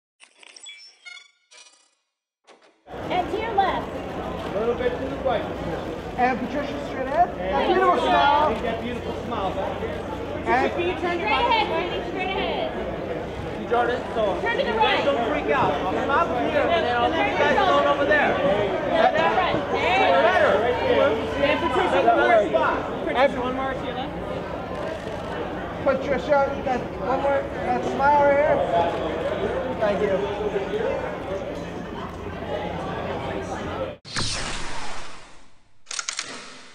Patricia Kazadi Kids' Choice Awards Orange Carpet Arrivals
BROLL: Actress and singer arrives at Kids' Choice Awards orange carpet held at The Forum in Inglewood, Ca March 12, 2016